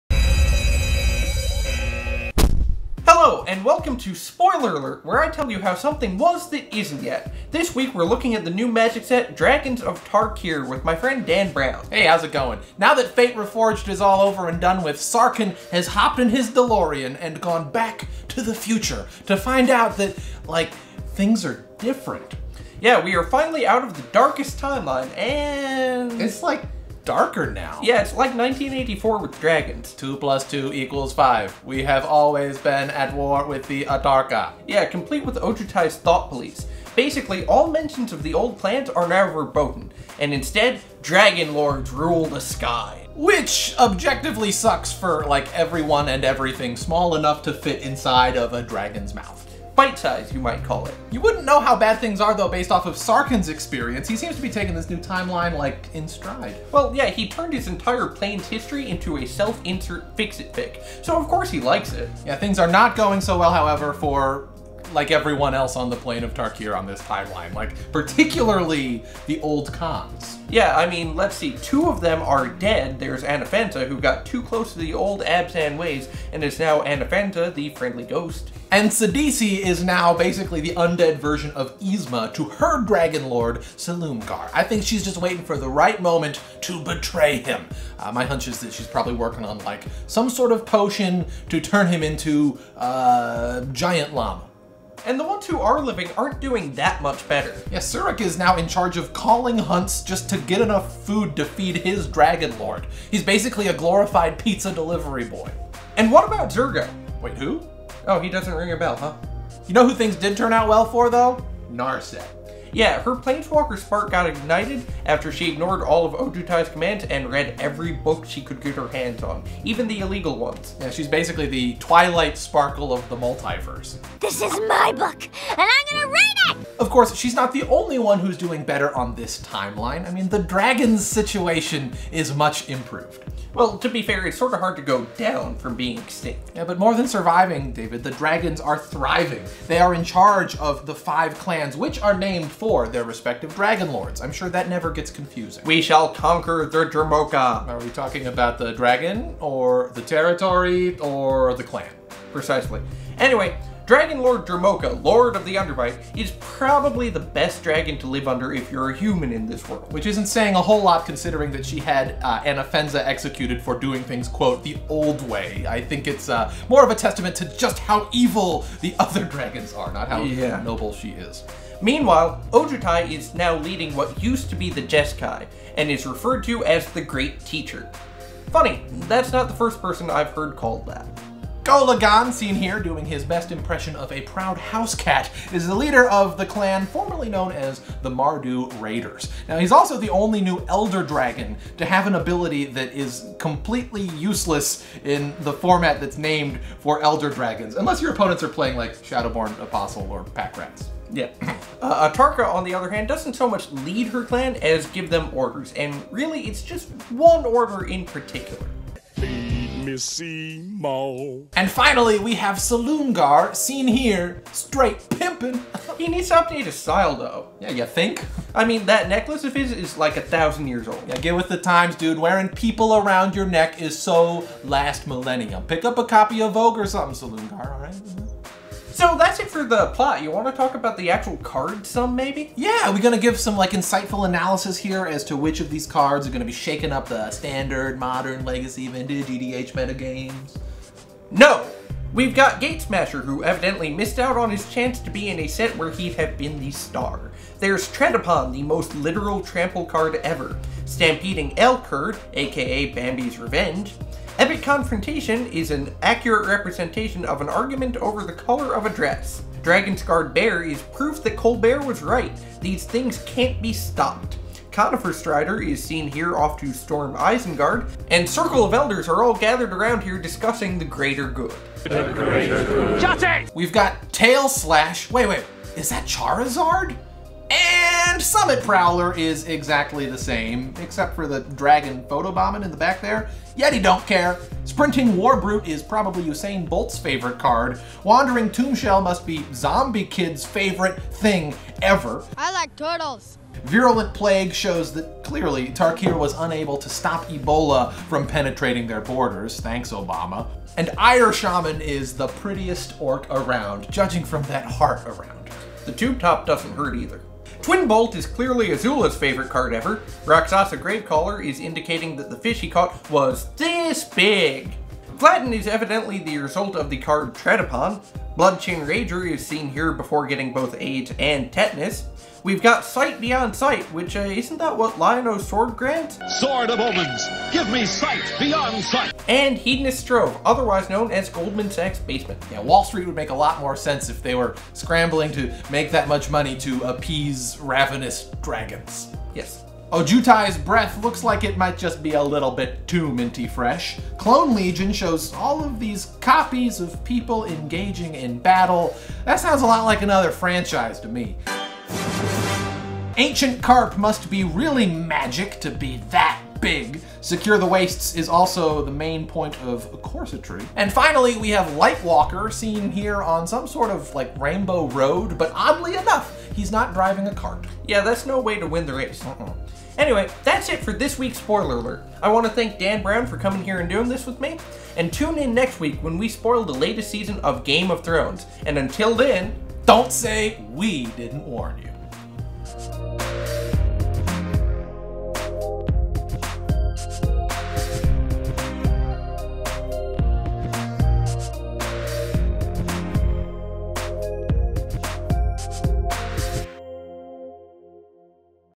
Dragons of Tarkir Spoiler Alert
Now that fate has been reforged, let's check up on how those dragons are doing on Tarkir.

Created by David Troth Wright and Ali Bjerknes, with help this week from Dan Brown